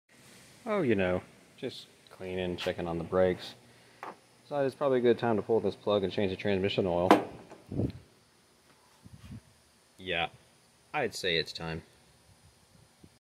mmmmmm, watery 80w90...
Argo Vanguard with a little water in the transmission.